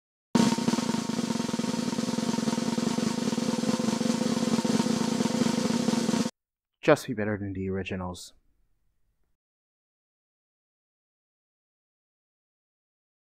How GameFreak can Make Diamond & Pearl Remakes the best in the Franchise
Let's have some fun and play some games with all my comrades.
This is Comedic Satire or a Joke please don't take it seriously